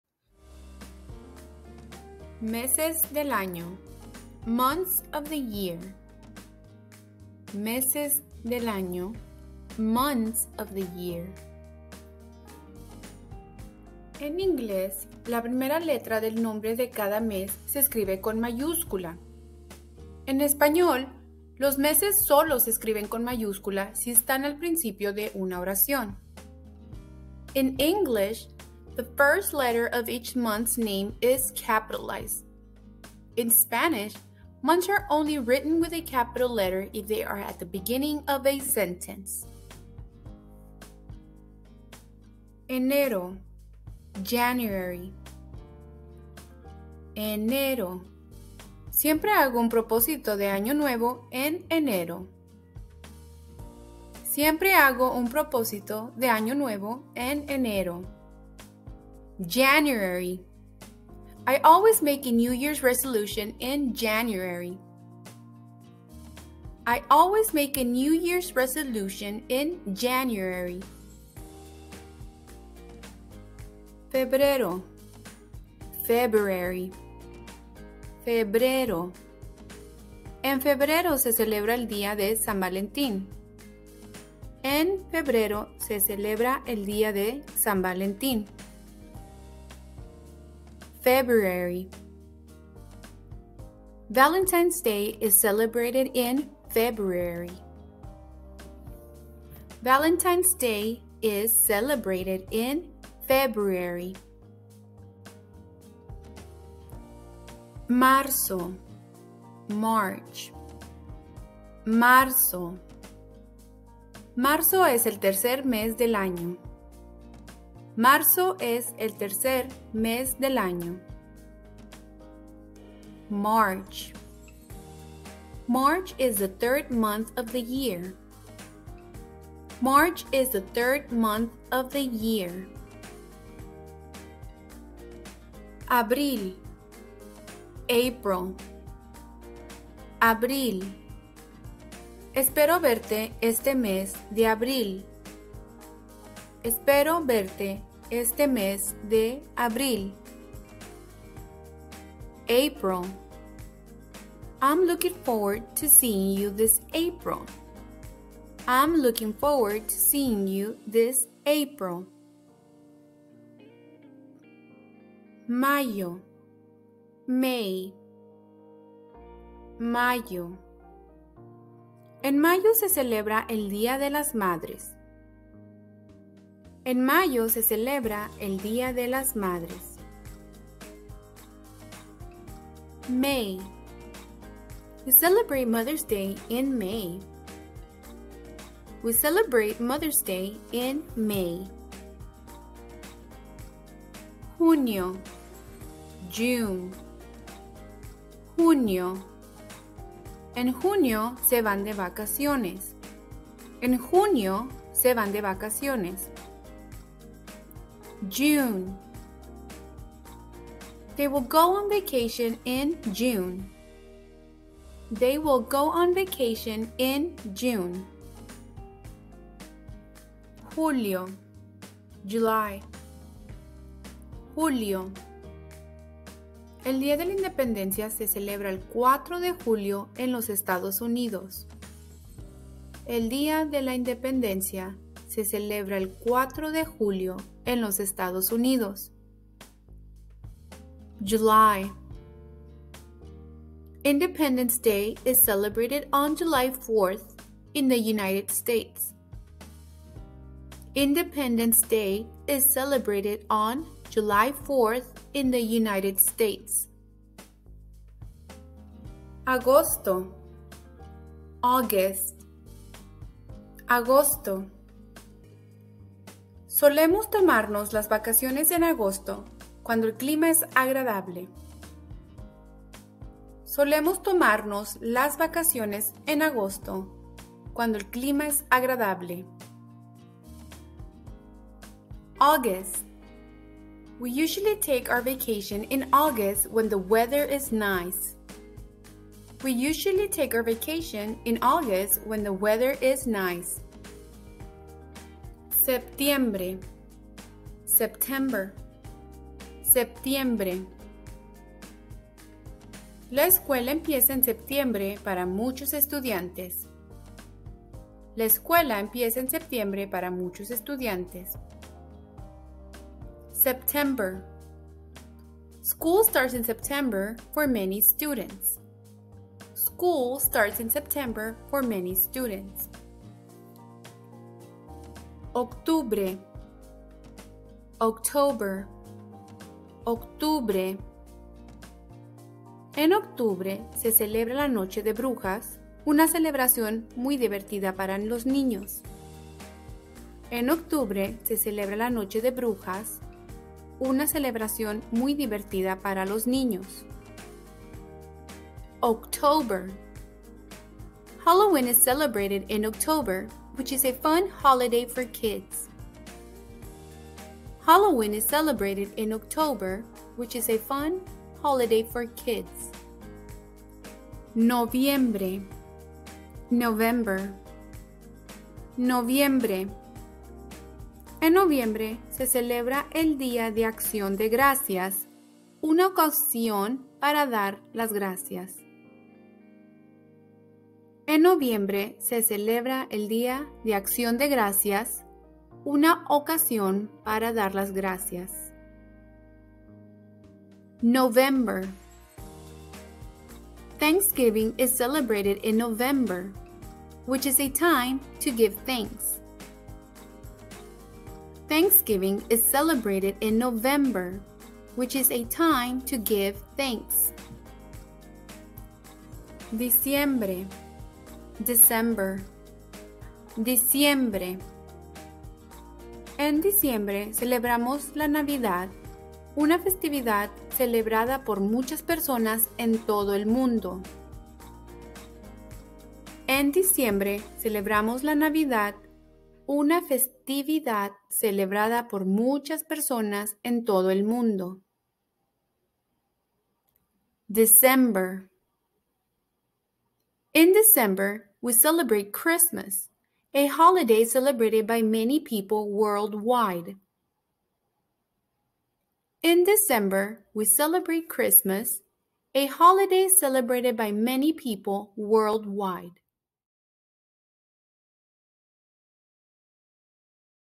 Aprender inglés y español Month of the Year Meses del año
Description: In this Learn English and Spanish video, we'll cover the topic of "Months of the Year- Los meces del año" We'll provide examples in both English and Spanish and explain how to use them in context. Whether you want to improve your English or Spanish language skills, this video has something for you.